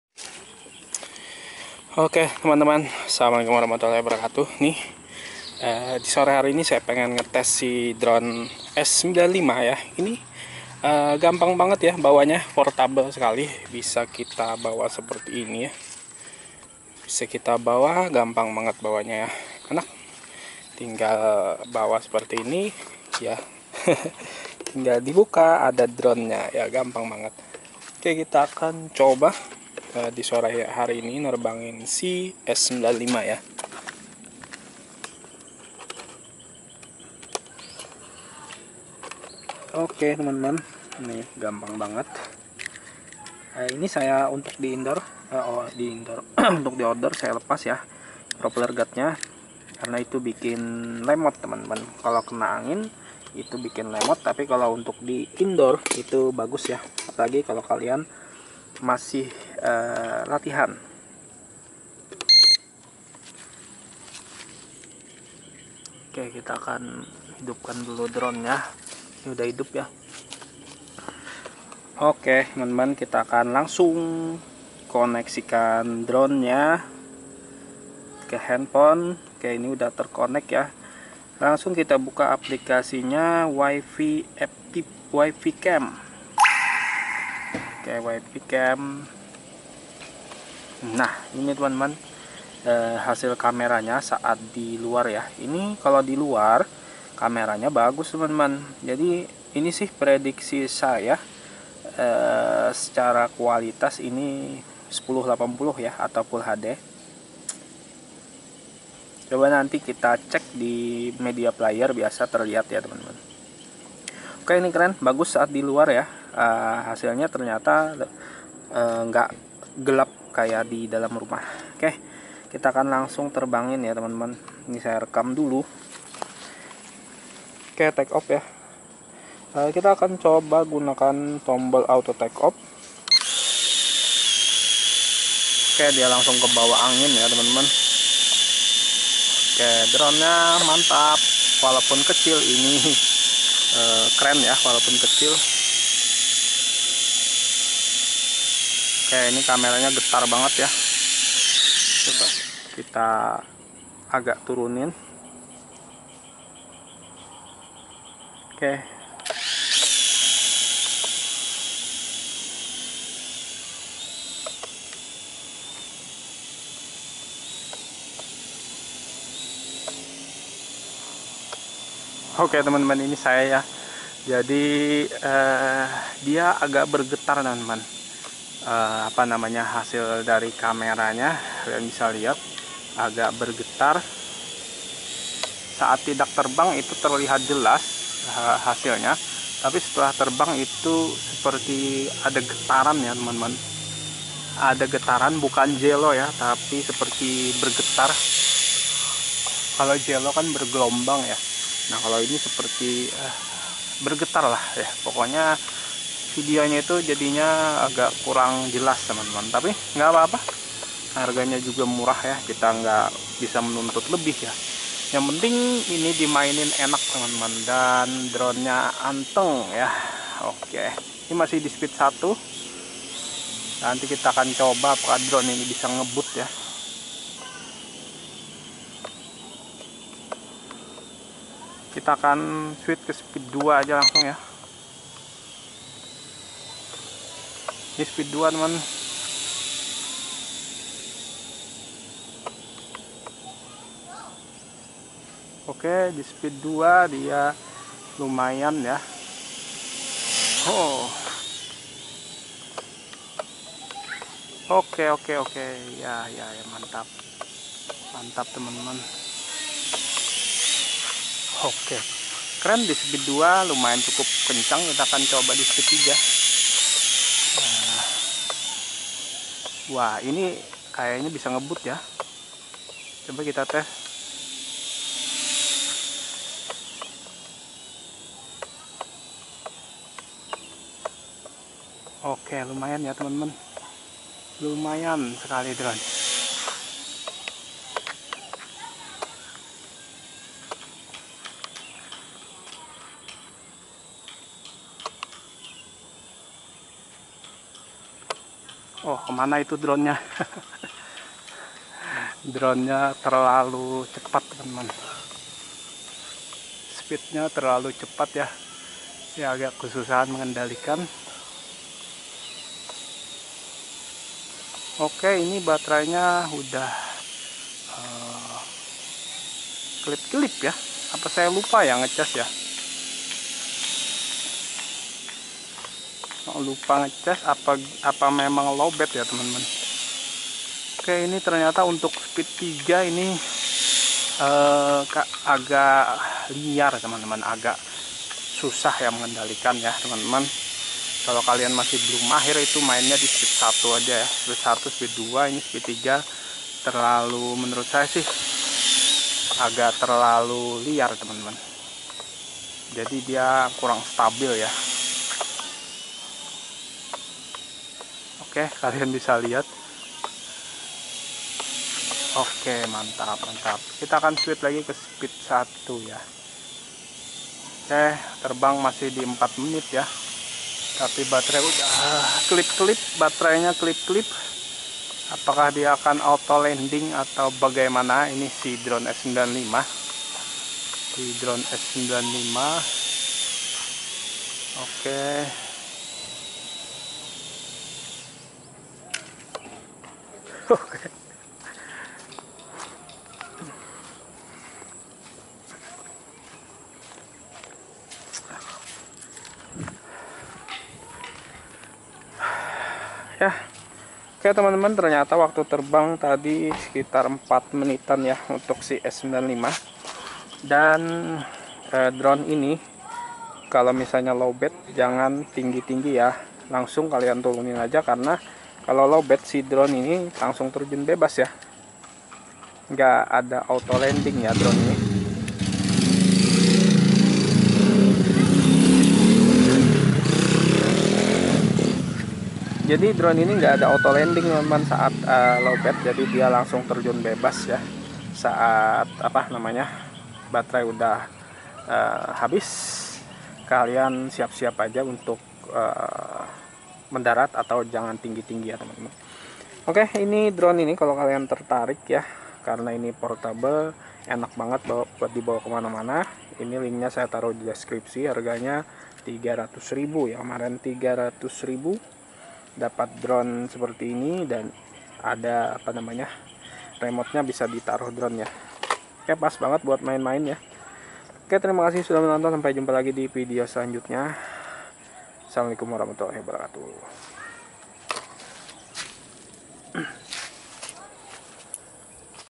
Tes terbang dan kamera drone mini S95
Drone S95 adalah drone murah 300 ribu berkamera yang bisa dimasukkan kedalam remotenya sebagai tempat penyimpanan dronenya, untuk pemula drone ini tidak disarankan menggunakan speed 3 ya guys,